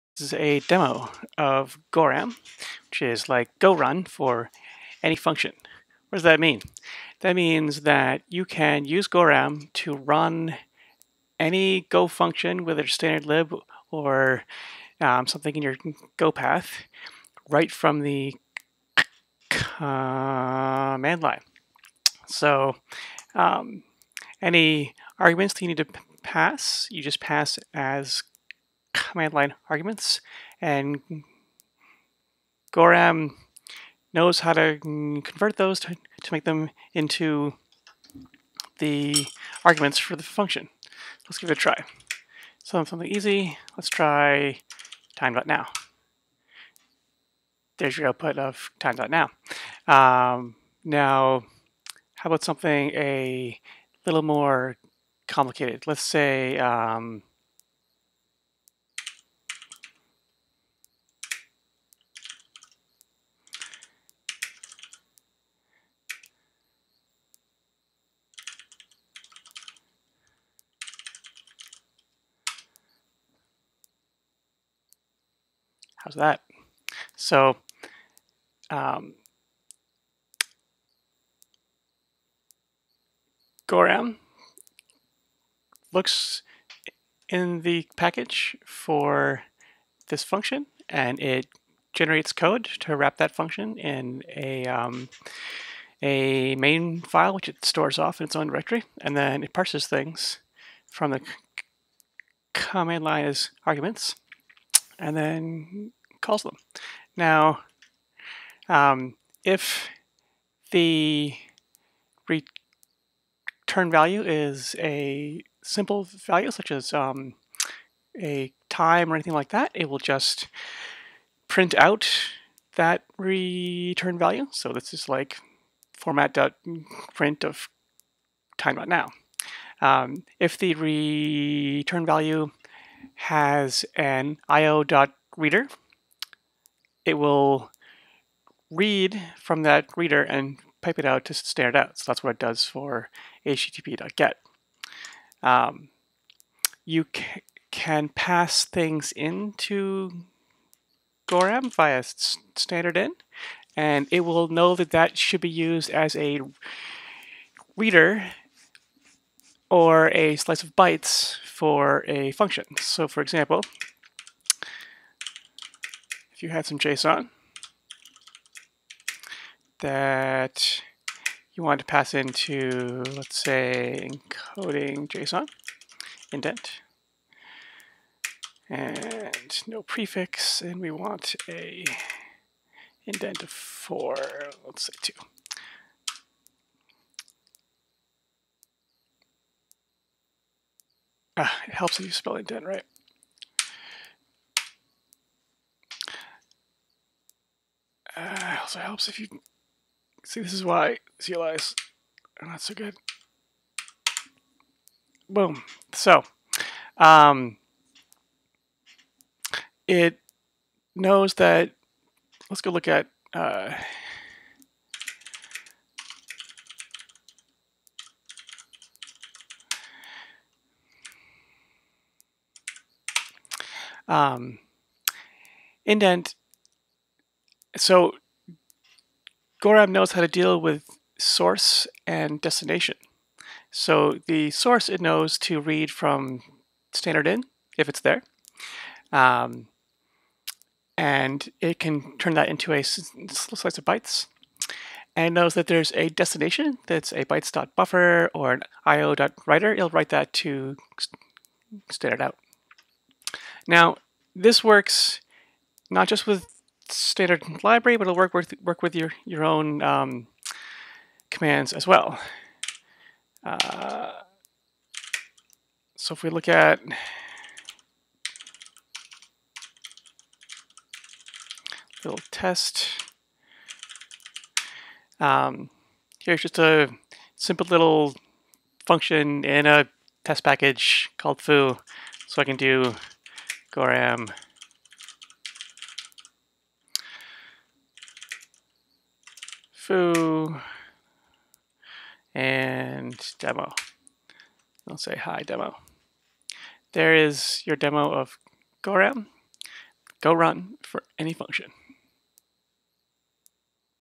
Demo of gorram, go run for any function
Gorram lets you run any standalone go function (or method on a exported variable) directly from the CLI, being super smart about translating files and stdin into io.readers and []byte and turning io.writers and byte buffers into output.